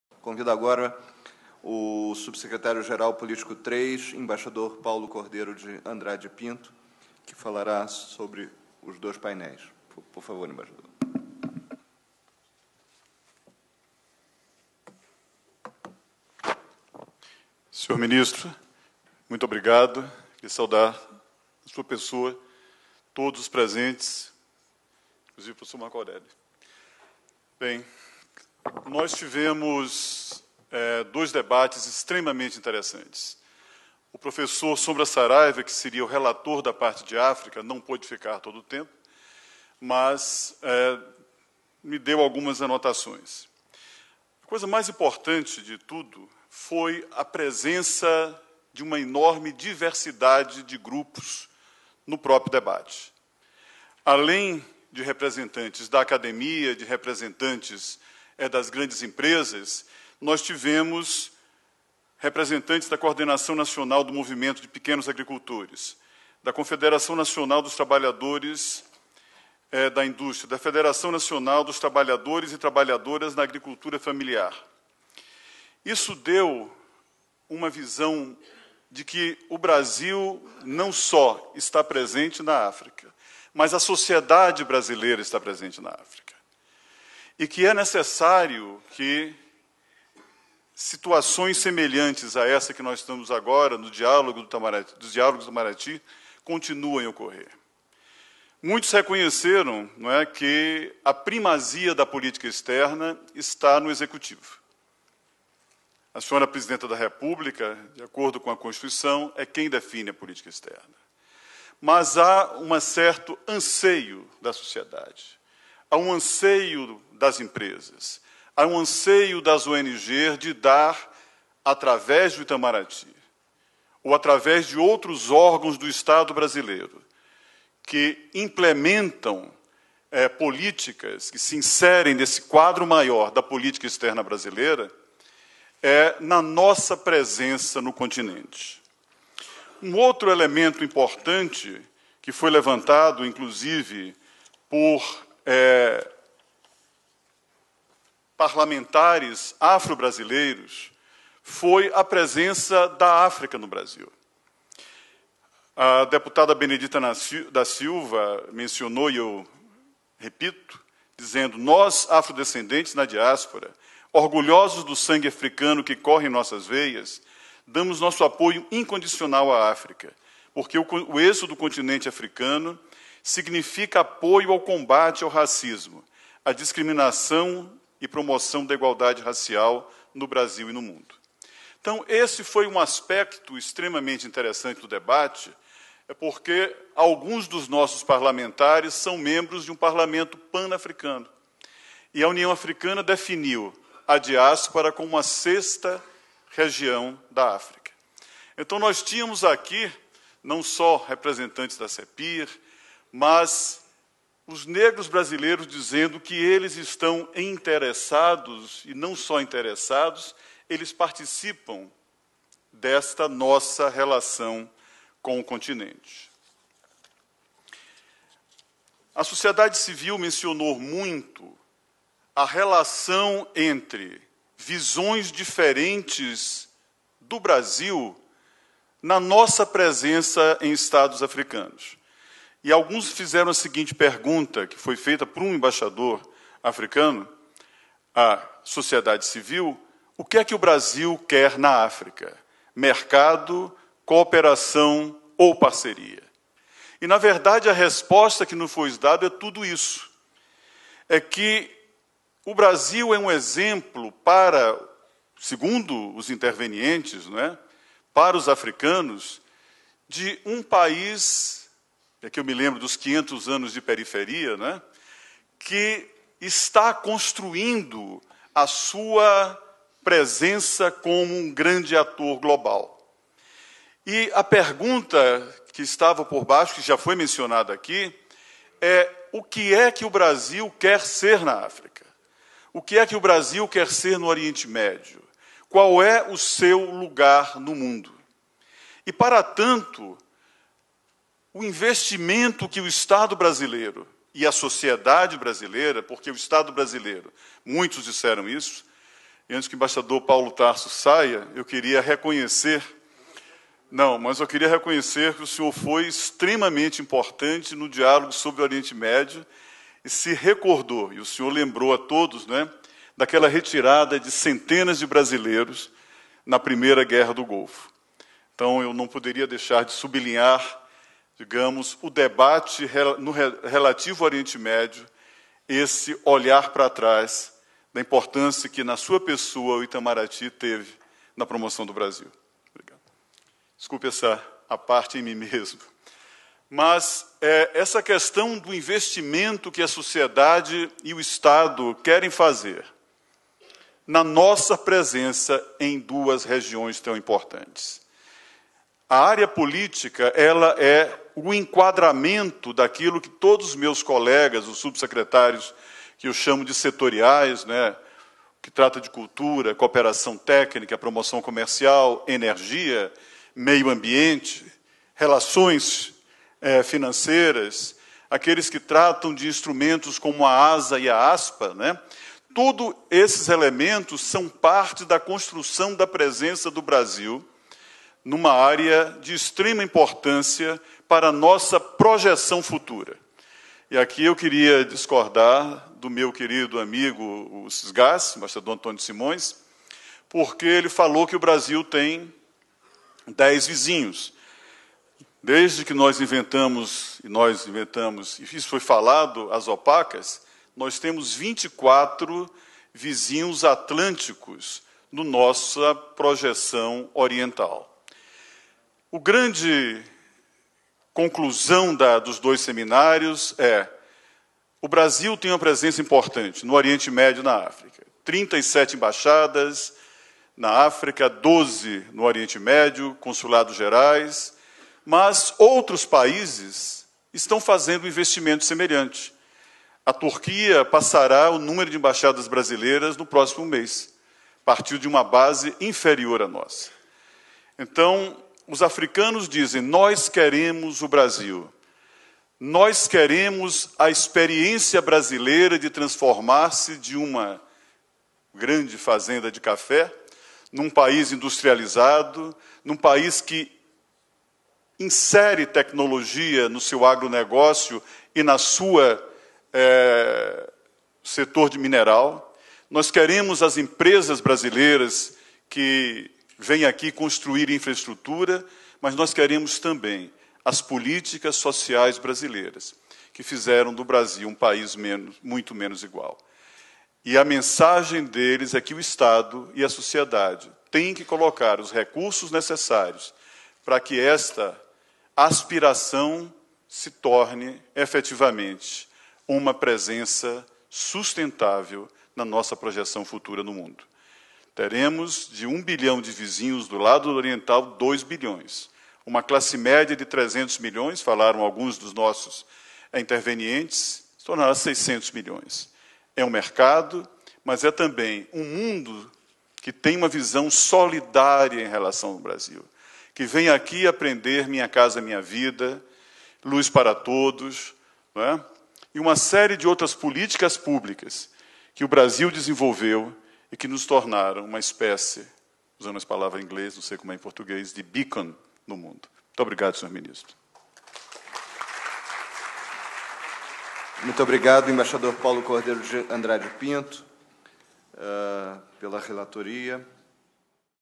Relatório do painel "África".
O Embaixador Paulo Cordeiro de Andrade Pinto, Subsecretário-Geral de Política III, apresenta o relatório final do painel "África".